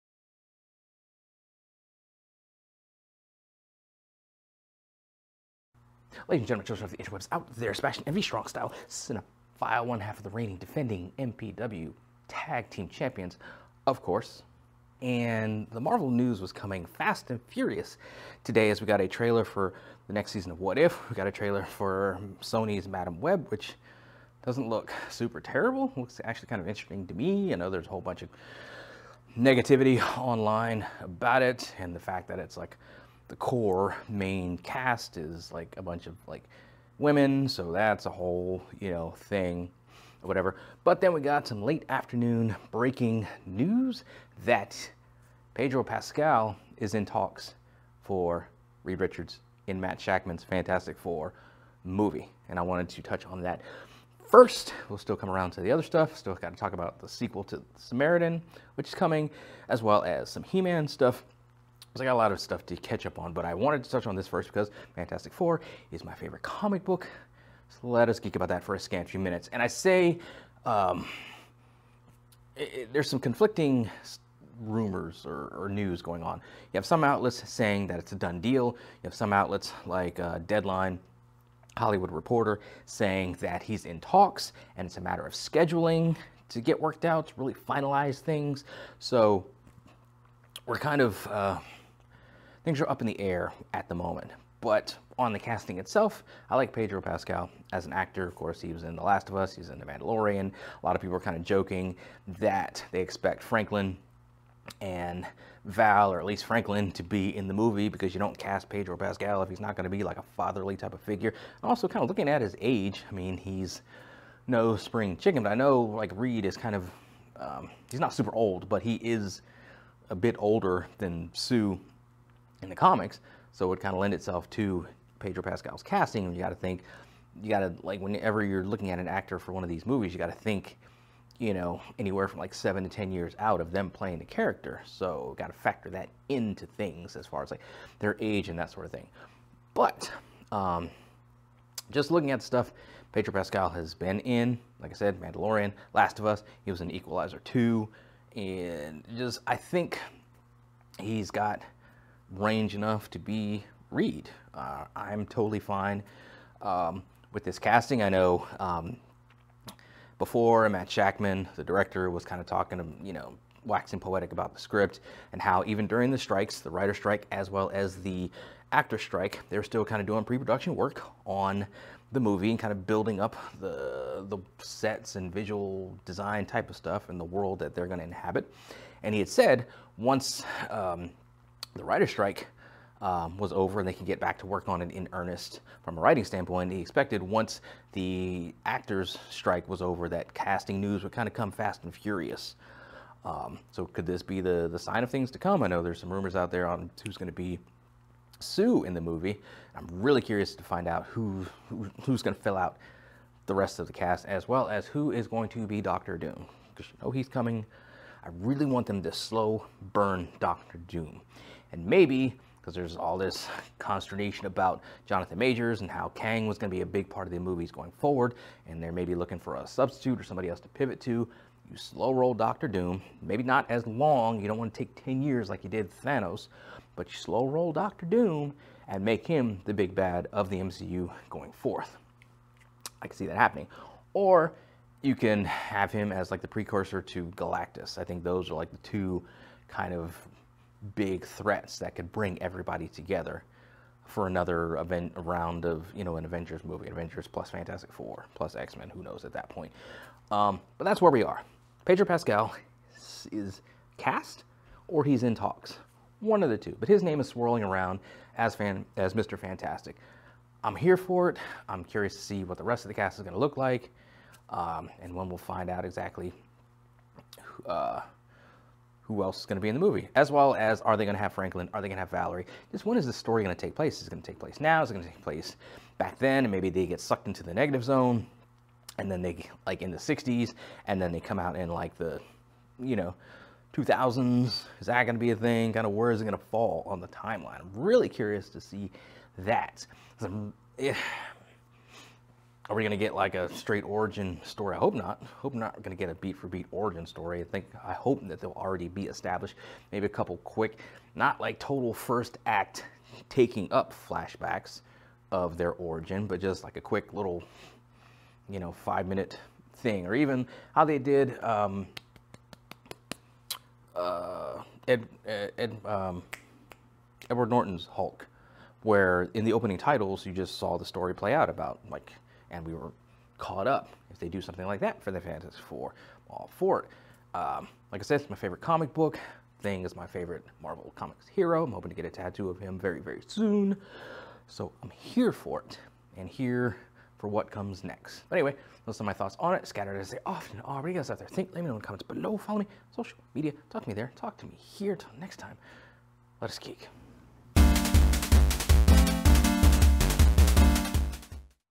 Pedro Pascal in talks to be MCU Mr. Fantastic!! ~ Let Us Geek
Yep...depending on where you read the story, Pedro Pascal is either in talks to play Mr. Fantastic or it's signed, sealed and delivered.

We shall see what happens.